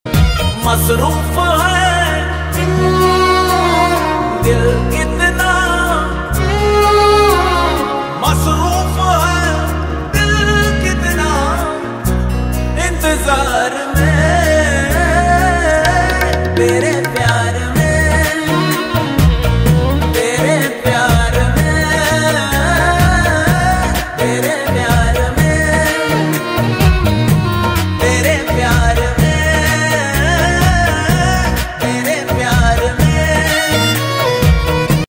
Unstitched suite material||Cotton Silk suite with plazo||Cash on delivery available
⭐Cash on delivery available
⭐3 Days return and exchange period

Message Ahmad_ai.boutique on WhatsApp

Kashvi Refined Salwar Suits & Dress Materials

Top Fabric: Cotton Silk + Top Length: 2 Meters
Bottom Fabric: Cotton Silk + Bottom Length: 2.25 Meters
Dupatta Fabric: Cotton Silk + Dupatta Length: 2 Meters
Lining Fabric: No Lining
Type: Un Stitched
Multipack: Single

cotton silk suite
cotton silk
ahmad boutique
cotton silk collection